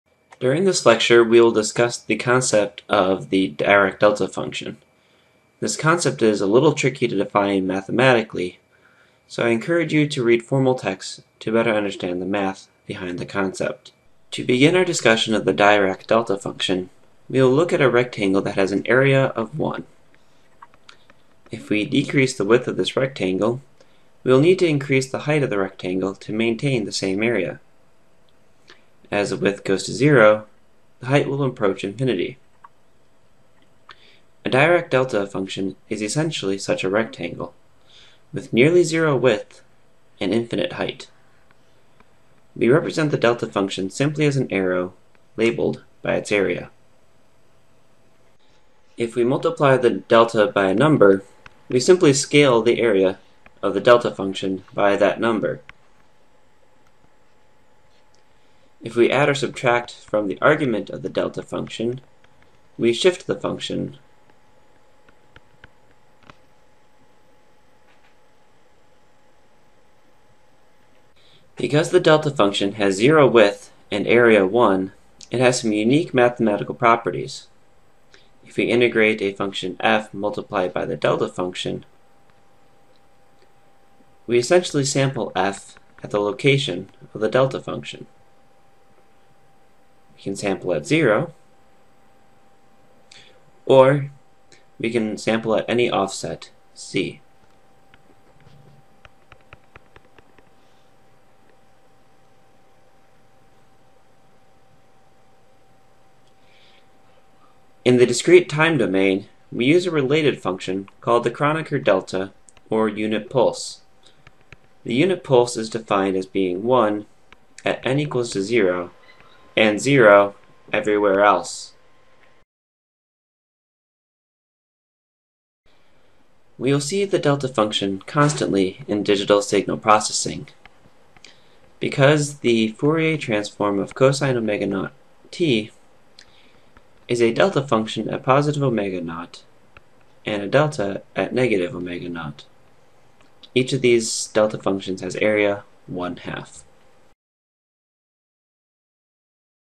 16 - Delta function
This lecture provides an overview of the delta function and some of its mathematical properties.

This lecture is adapted from the ECE 410: Digital Signal Processing course notes developed by David Munson and Andrew Singer